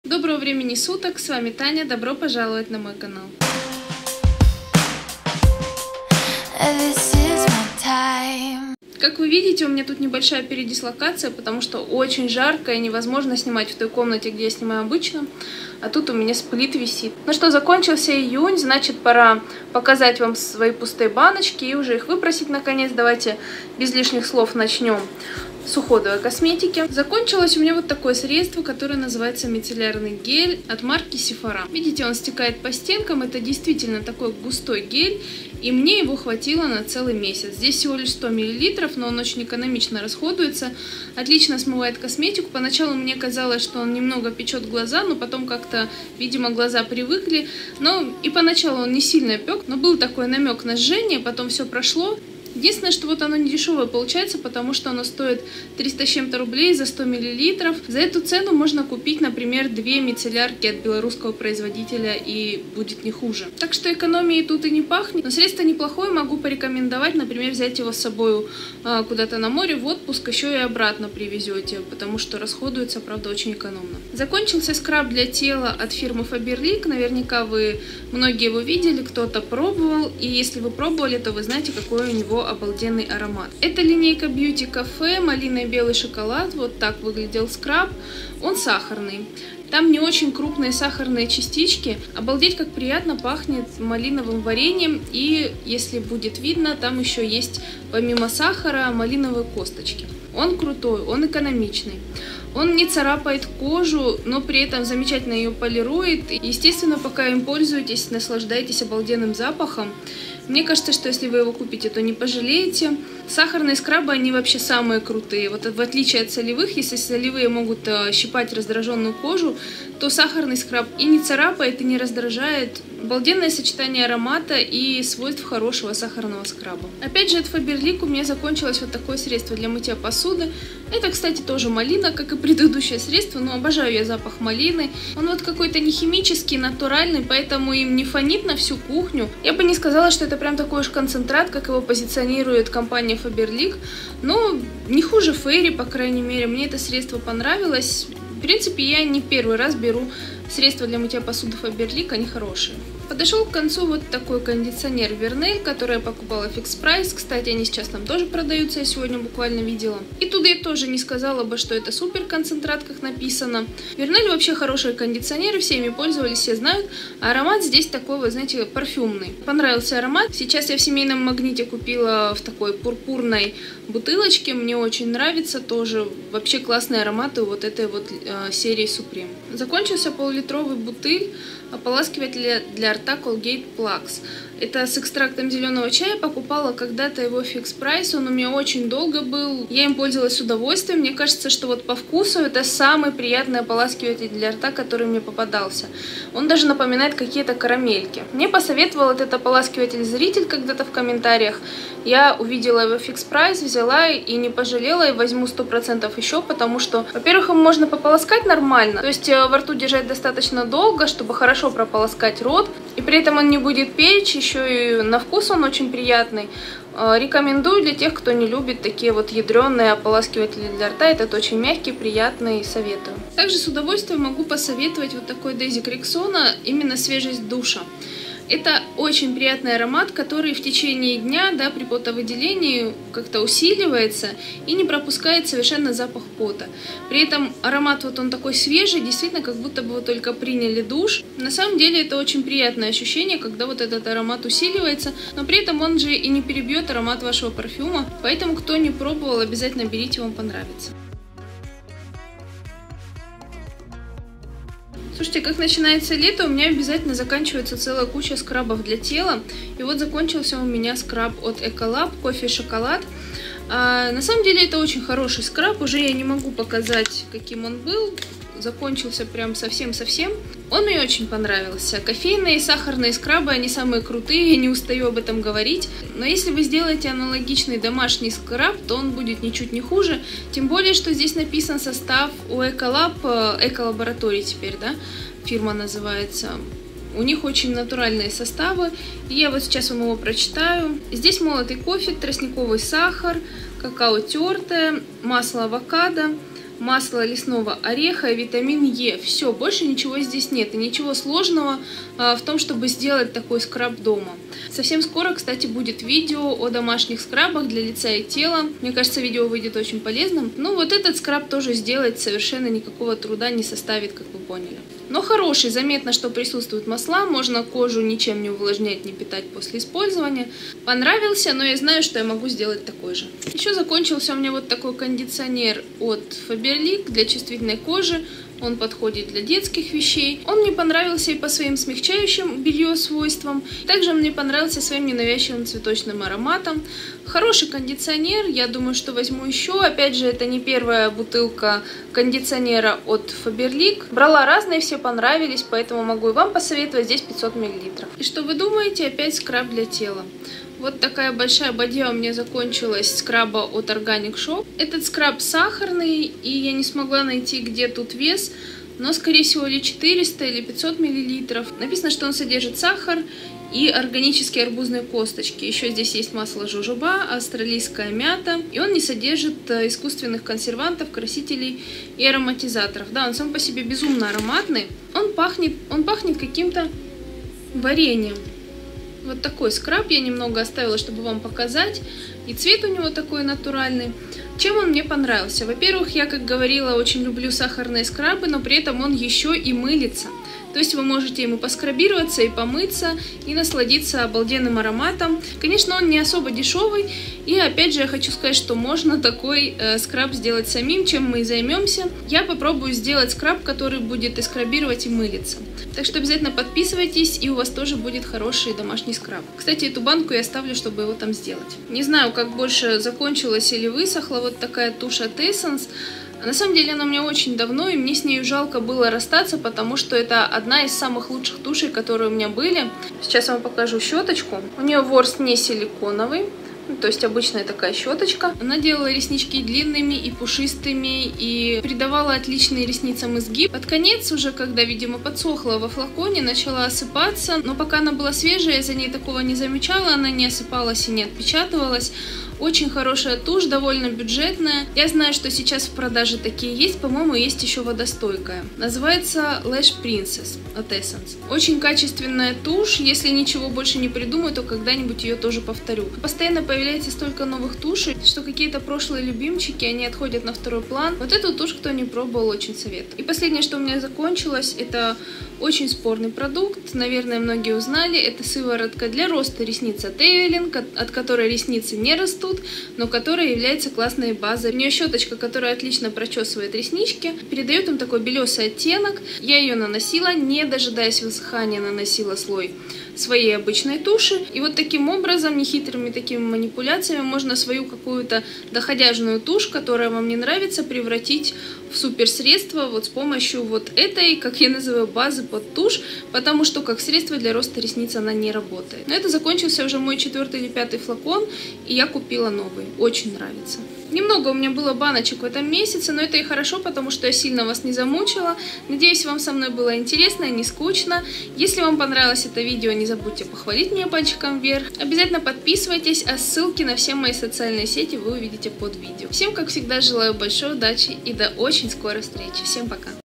ПУСТЫЕ БАНОЧКИ ИЮНЬ 2018 / КУПЛЮ ЕЩЁ! / МНОГО СКРАБОВ / БЫТОВАЯ ХИМИЯ / КЛАССНАЯ ТУШЬ
ПУСТЫЕ БАНОЧКИ ИЮНЬ 2018
Подошёл к концу июнь и вместе с ним некоторые средства)) В этом видео уходовая косметика, бытовая химия и декоративка!